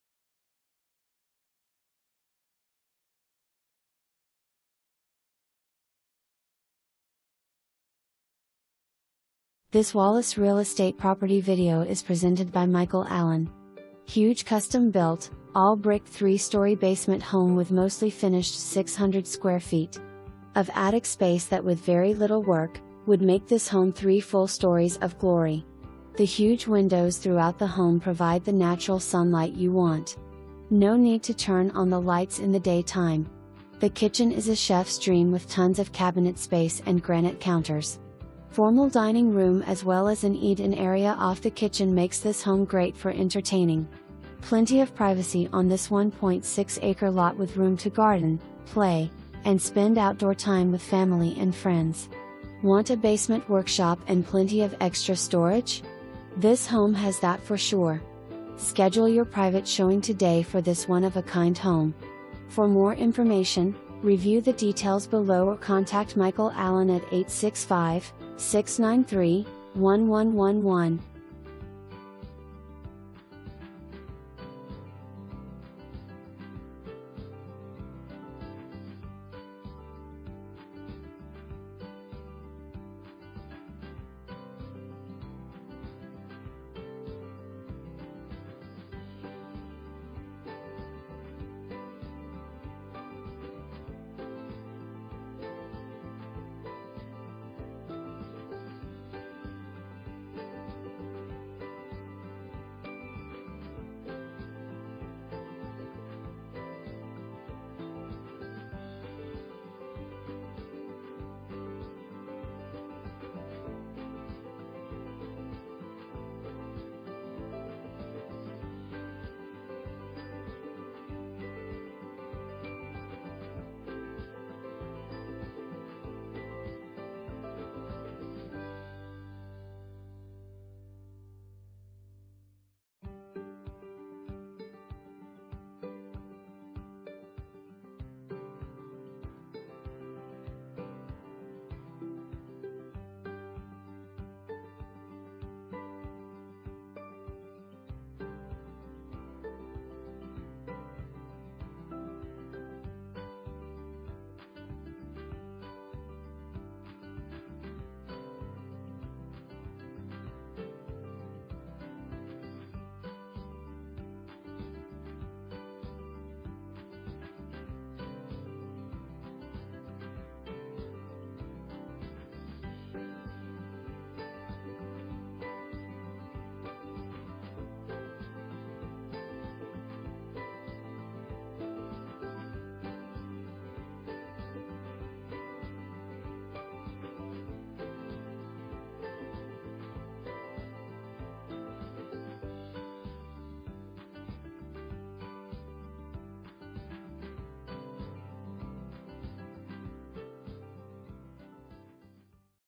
Wallace Real Estate - 4914 Circle Rd
Huge custom built, all brick 3 story basement home with mostly finished 600 sq. ft . of attic space that with very little work, would make this home 3 full stories of glory. The huge windows throughout the home provide the natural sunlight you want. No need to turn on the lights in the day time! The kitchen is a chef's dream with tons of cabinet space and granite counters. Formal dining room as well as an eat in area off the kitchen makes this home great for entertaining. Plenty of privacy on this 1.6 acre lot with room to garden, play, and spend outdoor time with family and friends. Want a basement workshop and plenty of extra storage? This home has that for sure! Schedule your private showing today for this one of a kind home!!

3 full bath

Michael Allen
Wallace Real Estate